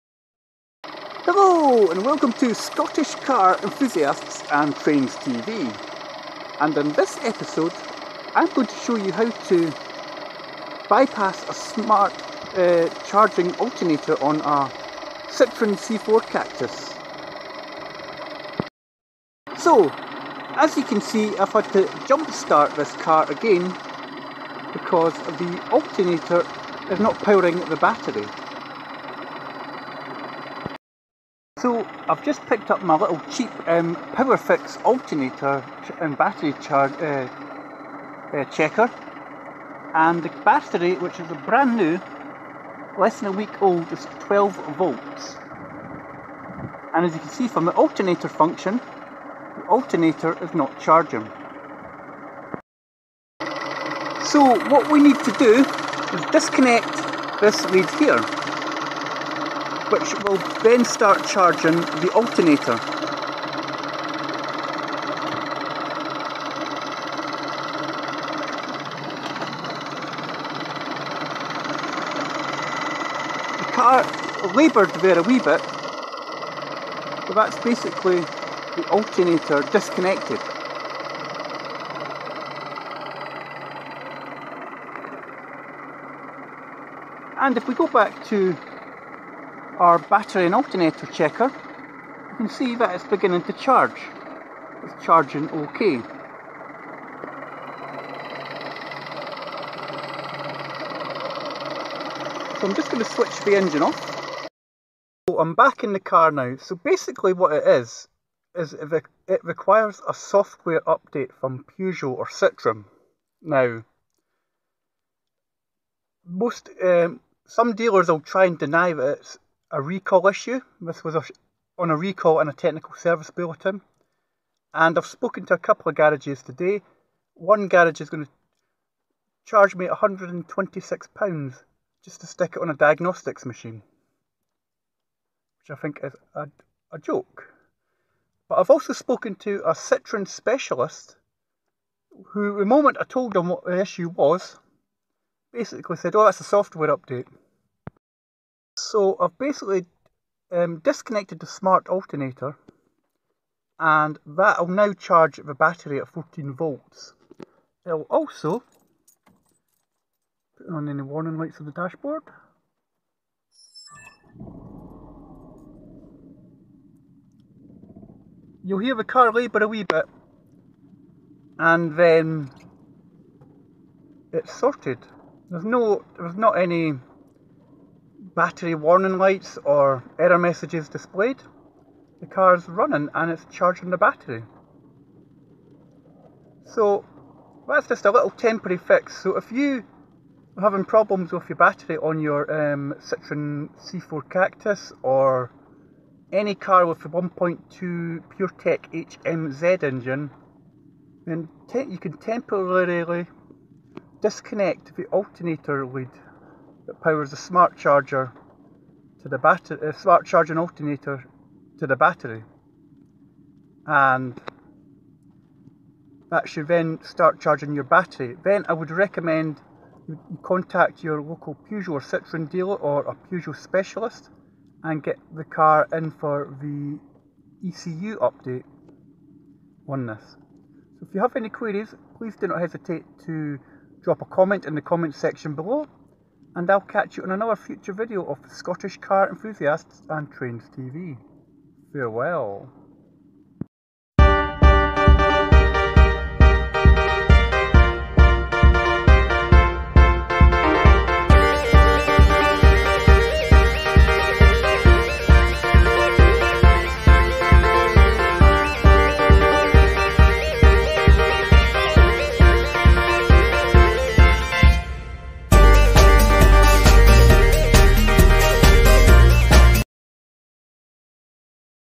Battery Charging Issues with Peugeot/Citroen 1.2 Puretech - Smart Alternator Fault
In this video I show you how to temporarily disable your Smart Alternator to allow your battery to charge on a 1.2 Puretech Peugeot or Citroen Engine.

The car in question is a 2016 Citroen C4 Cactus which recently had a flat battery, assuming the battery was old I ordered a new battery and had the same issue after replacing the battery.

With my friend @Teribus13 who is a mechanic we measured the voltage on the battery and alternator and was getting 12 volts, with the alternator disconnected we were getting 14 volts from the battery and alternator, it puzzled us till I found a couple of videos on a Peugeot 208 where I found out it it was a Smart Charging Alternator and it requires a ECU update to tell the alternator to charge the battery.

If you have this issue you may be able to get this done free of charge under a recall or TSB that Peugeot had, but I could not find anything for Citroen, but persist with the Peugeot/Citroen Dealer as I beleive some will try and deny this recall exists and charge you money to get this resolved either by telling you to get a new alternator or battery when all it requires is a ECU update.

After reading forums the general consensus is you can drive with the alternator plug disconnected but to get the software update carried out.